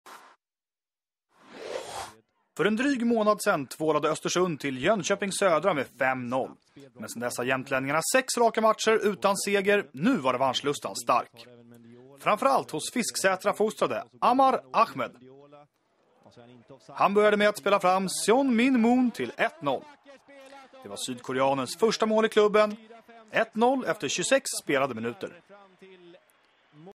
Seon-Min Moon Scores His First Superettan Goal
South Korean Seon-Min Moon scores for Östersunds FK.